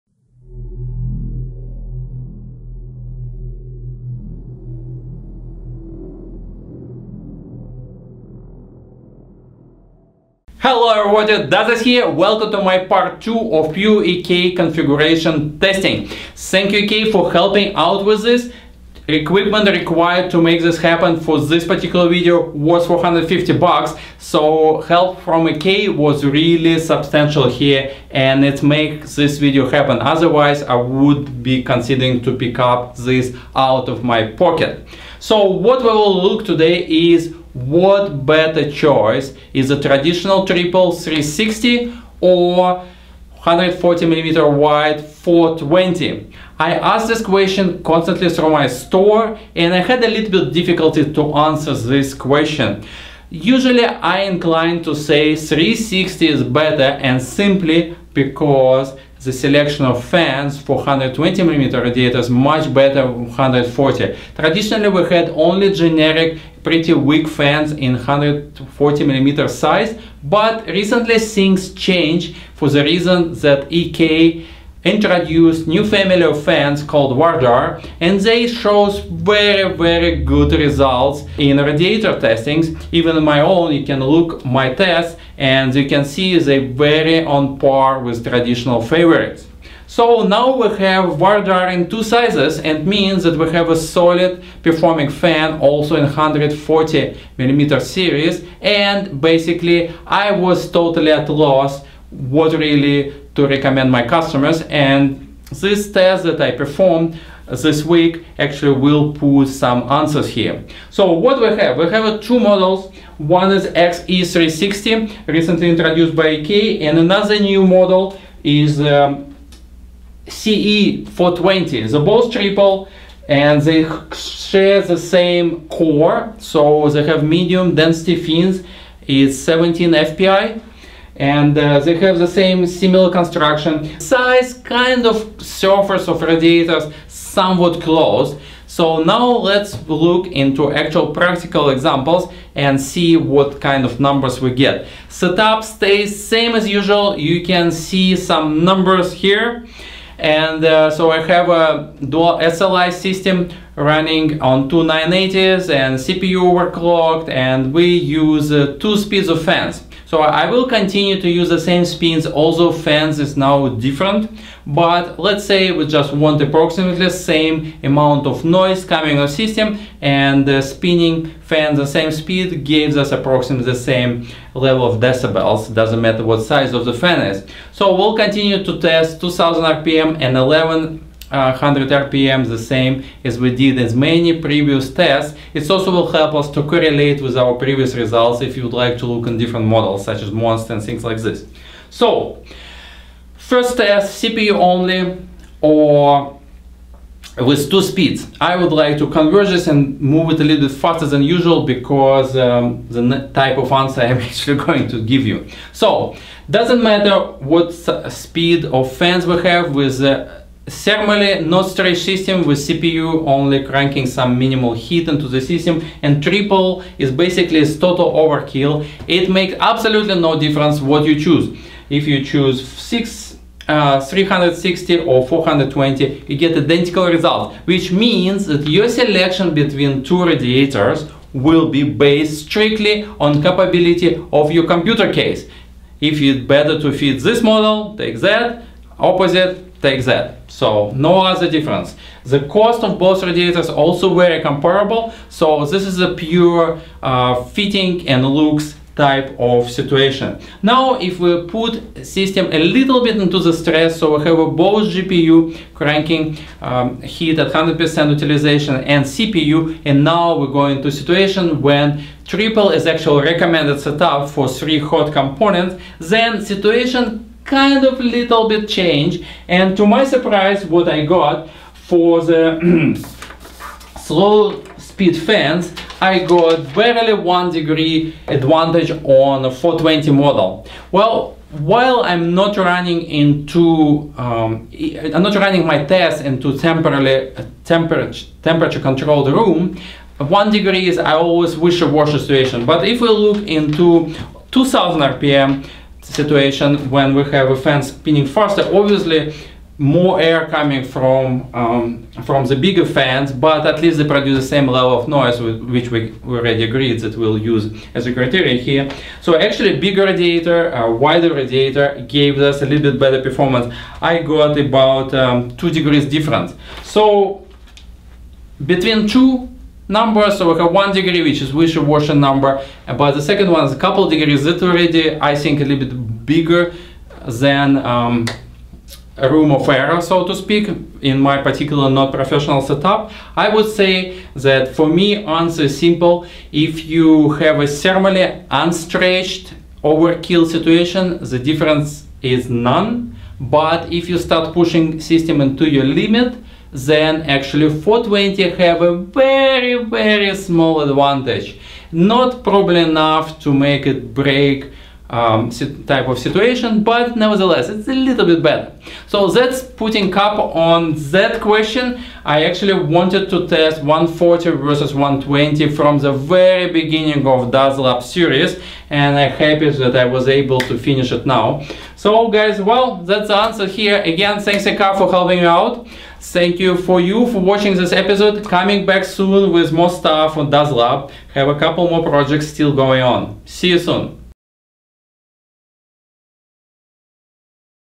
EK CoolStream: CE420 vs XE360 (120mm vs 140mm) -Dazlab 29
Looking into performance of very similar radiators for two different fan sizes: 120mm and 140mm.  Which is better better performer?